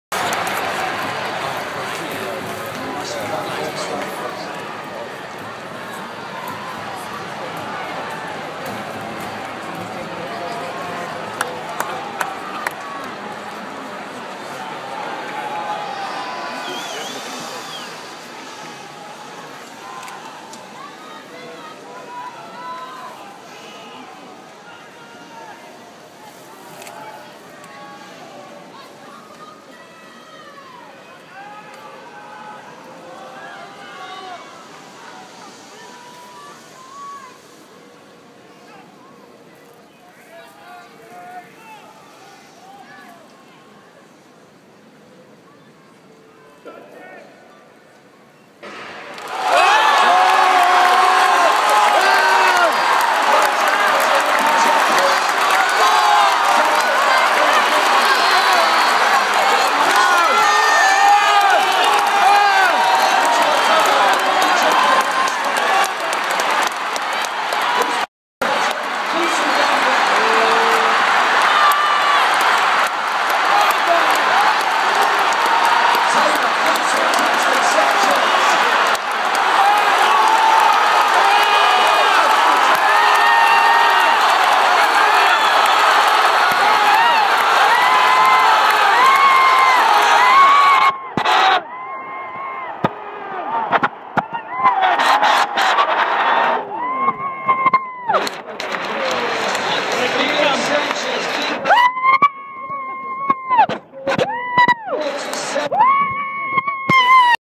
Dominicano Sanchez gana Oro en 400m vallas
The moment I saw our countryman win at the stadium. I went crazy !!